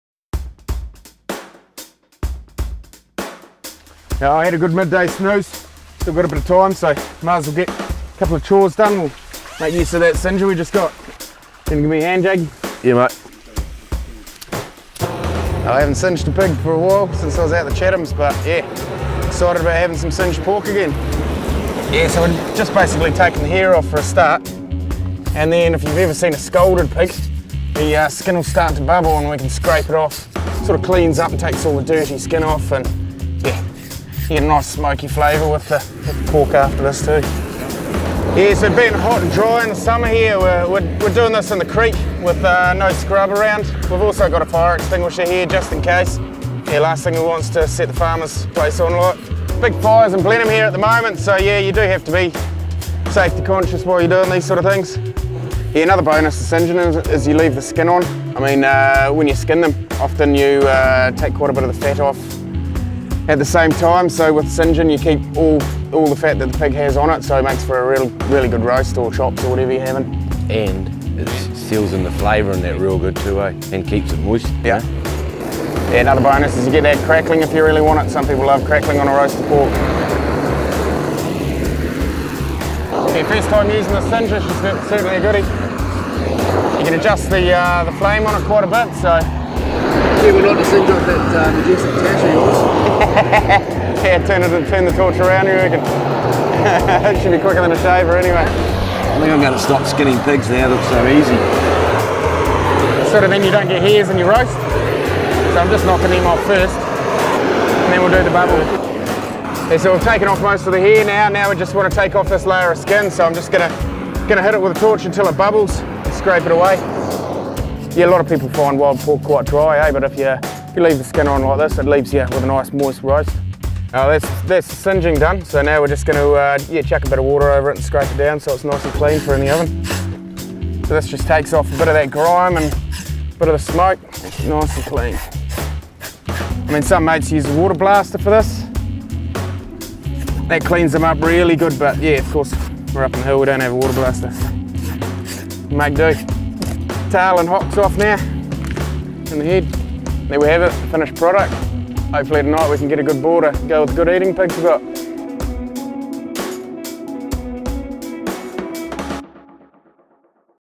HUNTERS CLUB - Singeing Masterclass
Tim tries out the Kovea Long Canon torch on a recent pig hunt in Marlborough